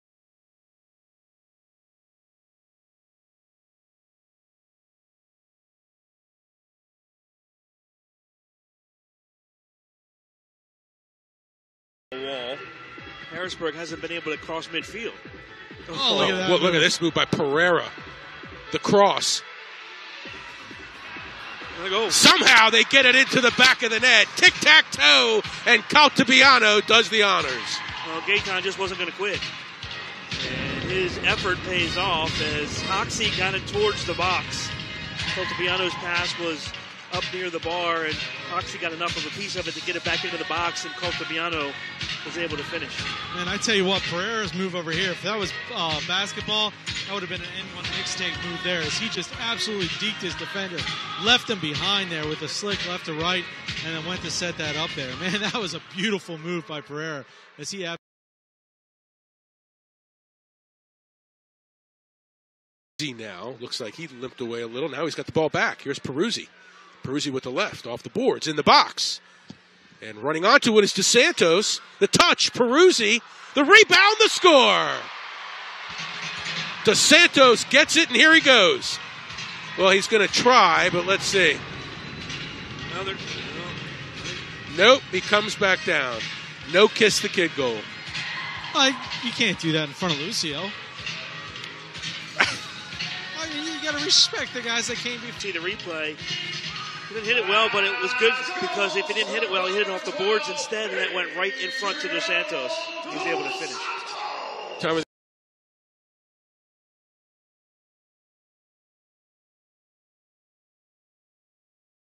Baltimore Blast Wallascora (2) 3-5-2017
#80 Geaton Caltabiano​ and #12 Adriano Dos Santos of the Baltimore Blast​ with a Wallascora​ against the Harrisburg Heat​ on 3-5-2017 in the Major Arena Soccer League​ on MASLtv YouTube​

Wallascora #1:  Assisted by Andrew Hoxie​
Wallascora #2:  Assisted by Daniel Peruzzi Gaguinha​